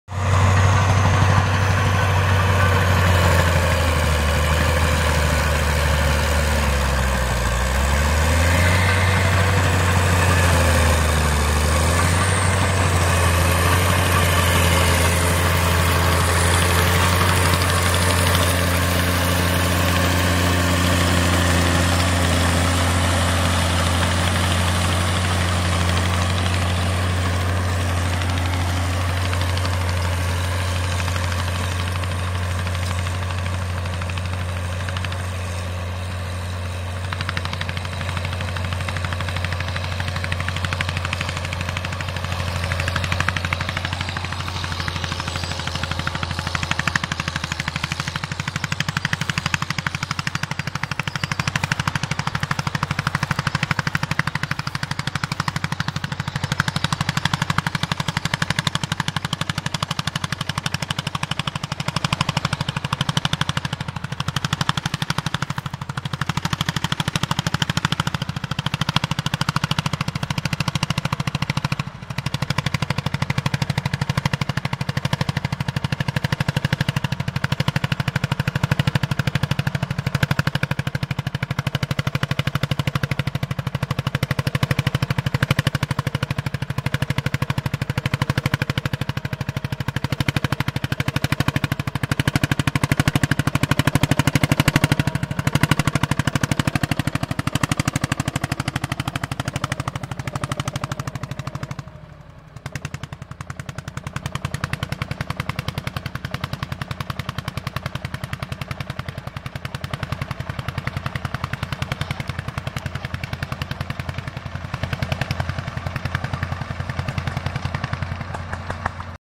Aratura storica 2018 cat in azione
Il cat d6c alle prese con terreno molto pestato con estirpatore